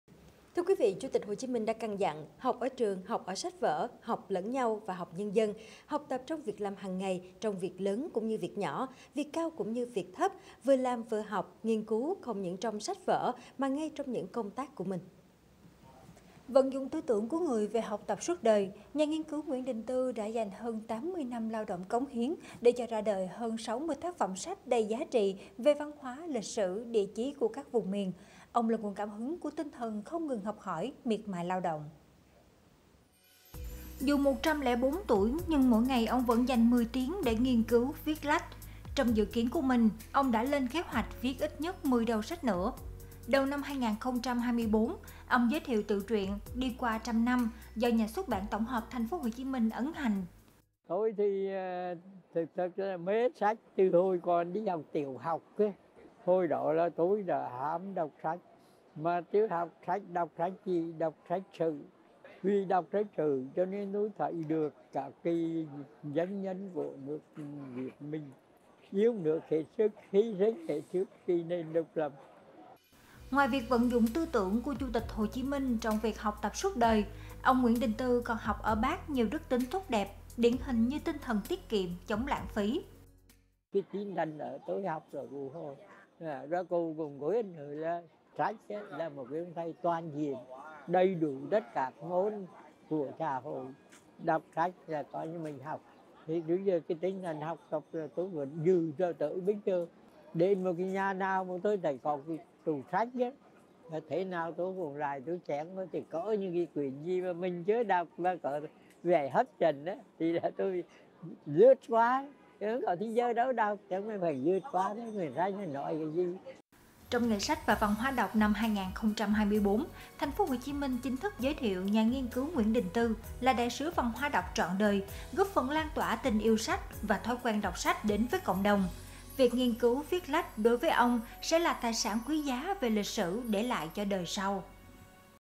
NHÀ NGHIÊN CỨU NGUYỄN ĐÌNH TƯ: TẤM GƯƠNG SÁNG HỌC TẬP SUỐT ĐỜI
Chủ tịch Hồ Chí Minh đã căn dặn: "Học ở trường, học ở sách vở, học lẫn nhau và học nhân dân; học tập trong việc làm hàng ngày, trong việc lớn cũng như việc nhỏ, việc cao cũng như việc thấp; vừa làm vừa học, nghiên cứu không những trong sách vở mà ngay trong những công tác của mình".